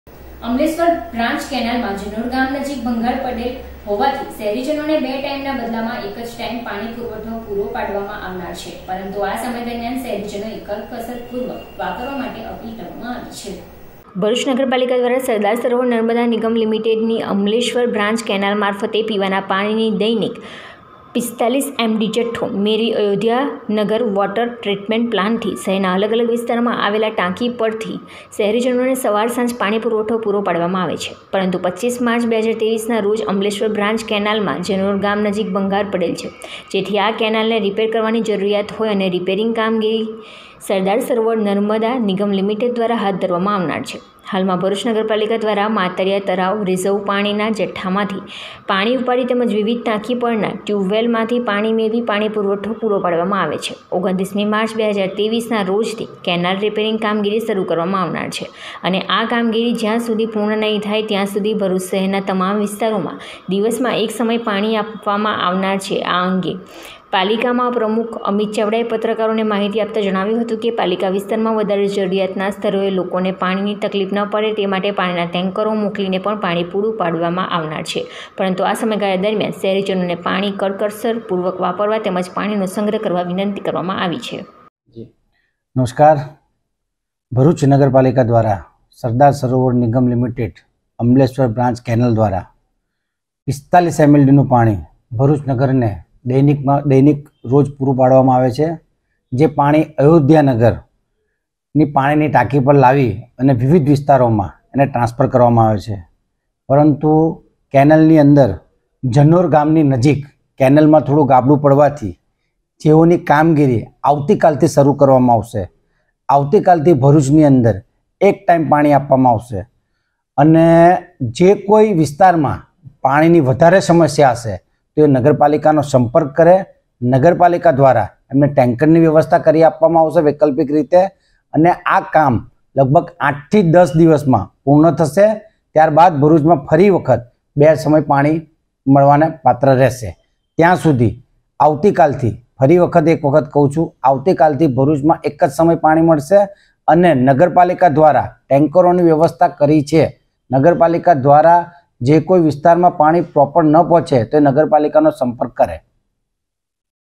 માઁ ટીવી 28. 03. 2023 બ્રાન્ચ કેનાલમાં ઝનોર ગામ નજીક ભંગાણ પડતા જળ સંકટ ઊભું થયું
જળ સંકટ : ભરૂચ શહેરમાં અમલેશ્વર બ્રાન્ચ કેનાલમાં ઝનોર ગામ નજીક ભંગાણ પડતા જળ સંકટ ઊભું થયું

 બે ટાઈમ ના બદલામાં એક ટાઈમ પાણી પુરવઠો પૂરો પાડવામાં આવશે : શહેરીજનોએ કરકસરપૂર્વક વાપરવા માટે અપીલ કરાઈ